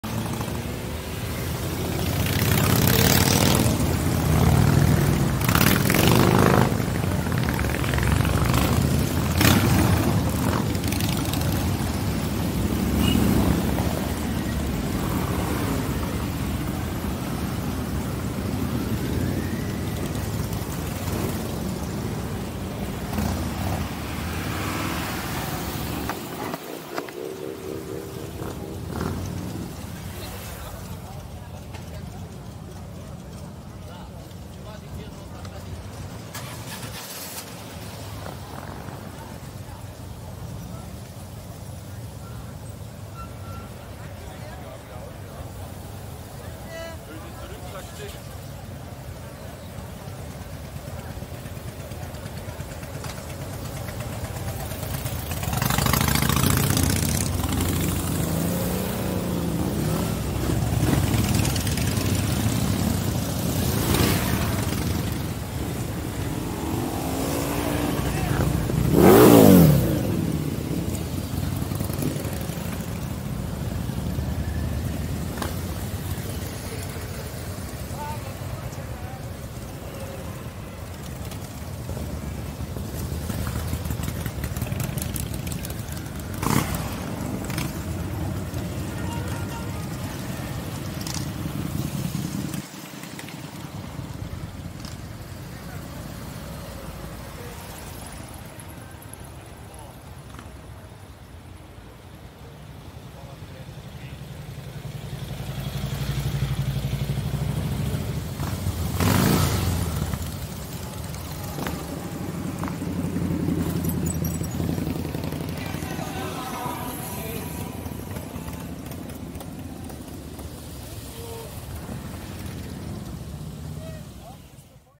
European Bike Week 2019 - 360 view INSTA 360 ONE X
Mi sono divertito ad andare al raduno a FAAKER SEE 2019 con la mia Harley Davidson Sportster 883 Iron - NOIS e utilizzare la mia INSTA 360 ONE X per fare riprese a 360° da condividere con tutti voi

⬇️LA MIA MOTO⬇️
Harley Davidson Sportster 883 Iron del 2009

⬇️LA MIA  ATTREZZATURA⬇️
Gopro Hero Black 4 & Gopro Hero Black 5 - Gimbal Feiyutech G6 - Gimbal Removu S1